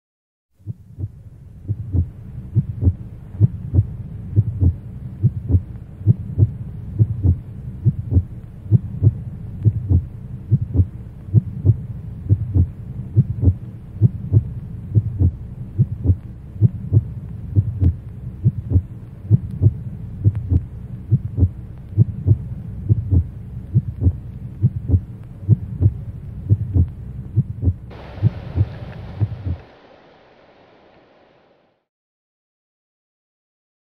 Spot Premio Nereide 2011
Spot realizzato per il progetto Premio Nereide 2011 - corso Management degli eventi e dei prodotti musicali, teatrali e cinematografici.